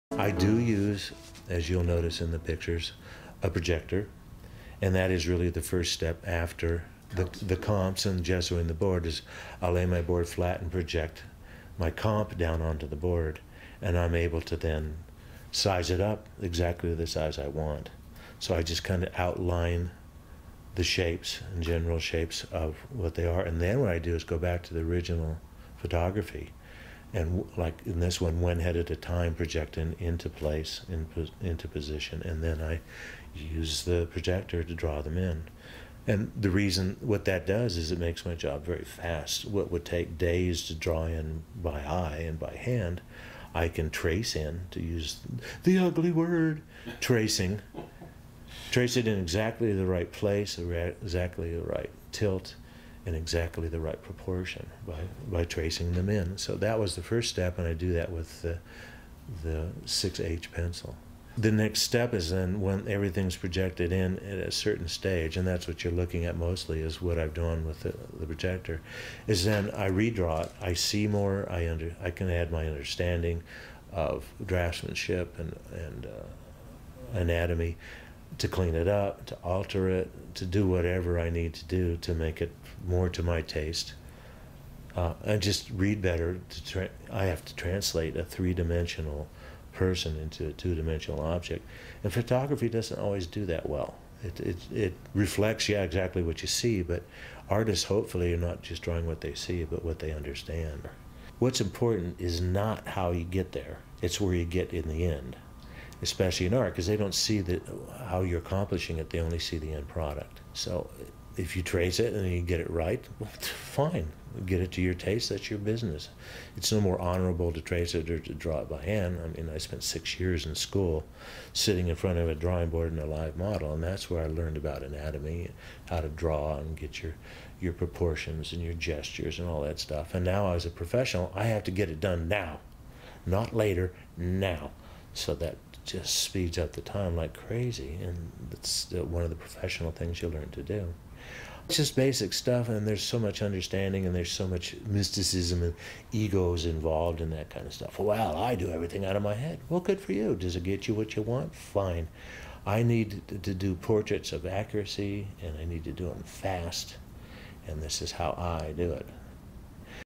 Drew Struzan - Tracing
Drew discusses tracing and creating art